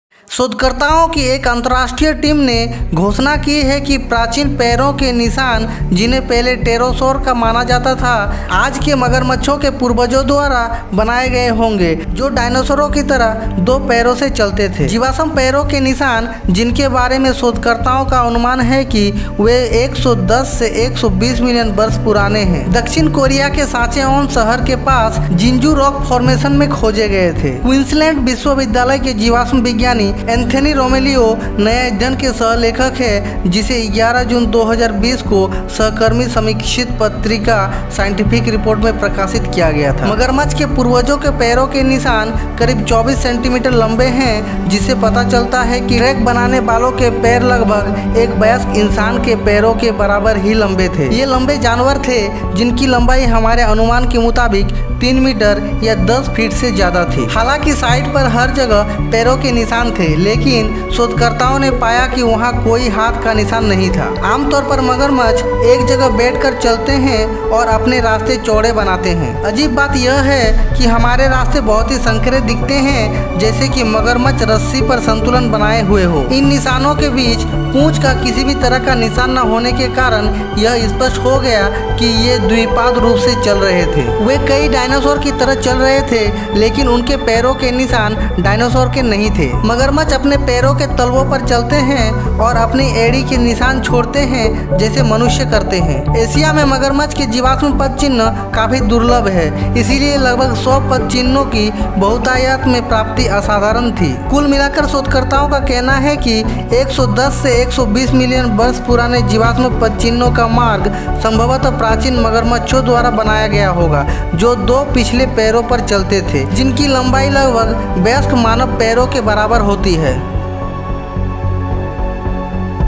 मगरमच्छ के पूर्वज कैसे दो पैरों से चलते थे how ancient crocodile walked on two leg
मगरमच्छ के पूर्वज कैसे दो पैरों से चलते थे
प्रागैतिहासिक मगरमच्छ कैसा दिखता है
how prehistoric crocodile look like
crocodile ancestor